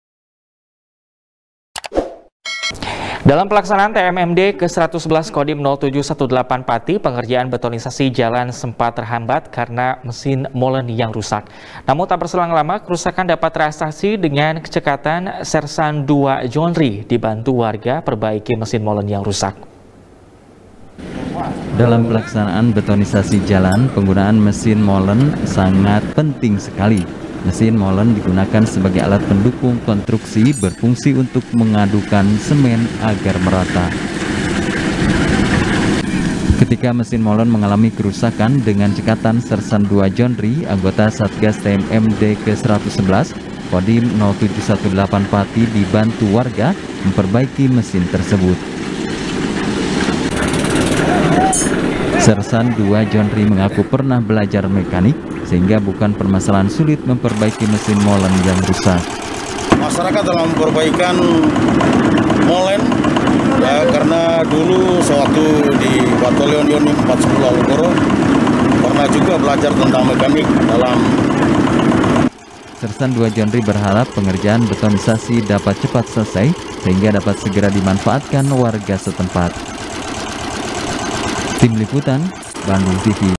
Satgas Perbaiki Mesin Molen Rusak Di Lokasi Tmmd | SBR | BANDUNGTV
SBR | BANDUNGTV | IRFAN RAMANDA | Dalam Pelaksanaan TMMD Ke-111,Kodim 0718 Pati, Pengerjaan Betonisasi Jalan Sempat Terhambat Karena Mesin Molen Yang Rusak.Namun Tak Berselang Lama,Kerusakan Dapat Teratasi,Dengan Cekatan Sersan Dua Jondri,Dibantu Warga Perbaiki Mesin Molen Yang Rusak.